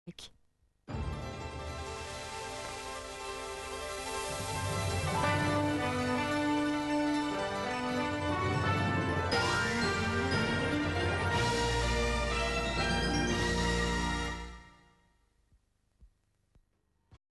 Sunshine Television ID 1991
An ID for Sunshine Television (now known as 7 Queensland) from early 1991.